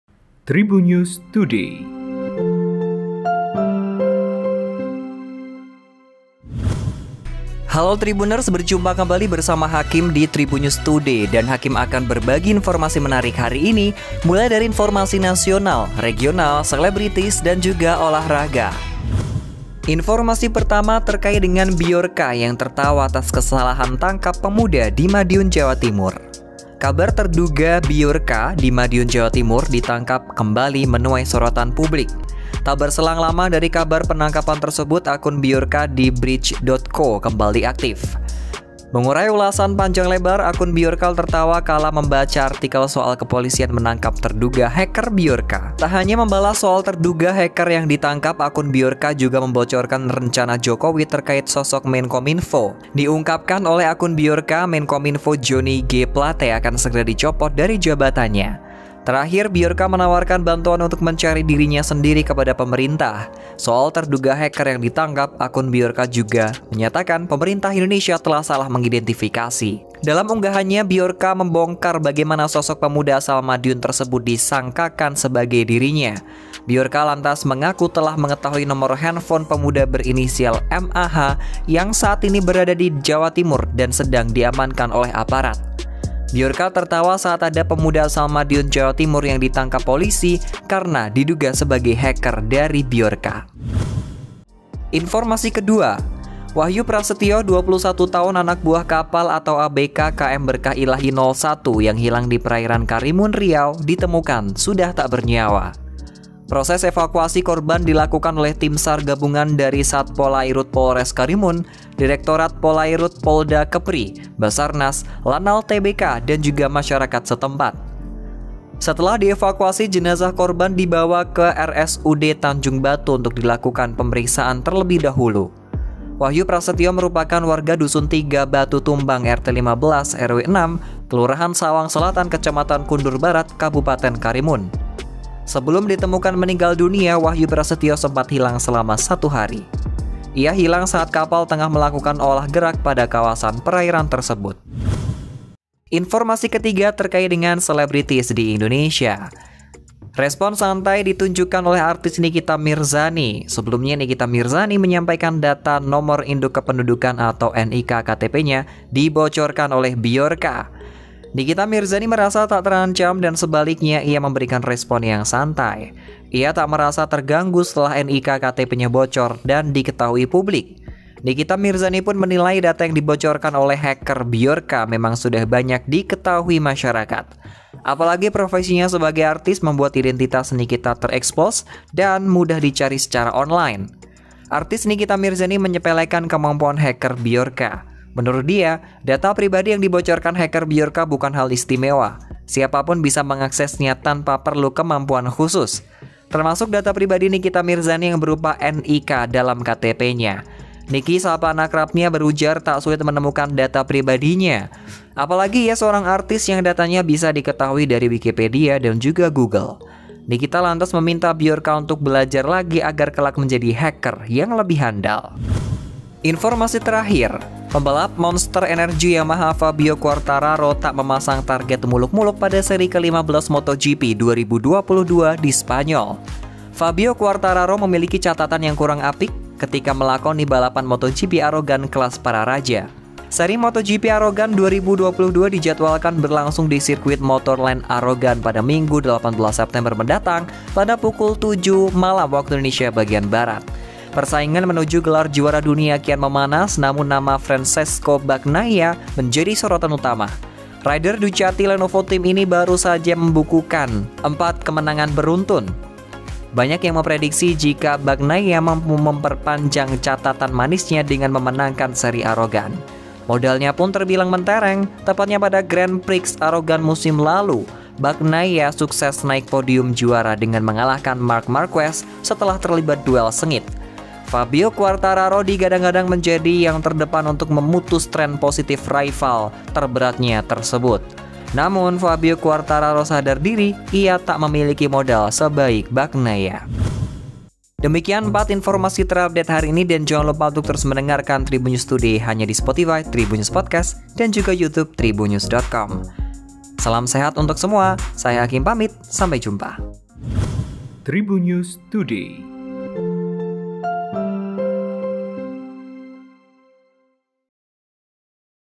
Bjorka VS Pemerintah Indonesia, NIK KTP Nikita Mirzani Bocor, Aksi Fabio di MotoGP Aragon 2022
TRIBUN-VIDEO.COM - Kabar terduga Bjorka di Madiun ditangkap kembali menuai sorotan. Tak berselang lama dari kabar penangkapan tersebut, akun Bjorka di breached.co kembali aktif.

Wahyu Prastyo (21), Anak Buah Kapal (ABK) KM Berkah Illahi 01 yang hilang di perairan Karimun, Riau ditemukan tak bernyawa.

Nikita Mirzani merasa tak terancam dan sebaliknya ia memberikan respons santai saat data NIK KTP nya bocor.

Simak selengkapnya Tribunnews Podcast di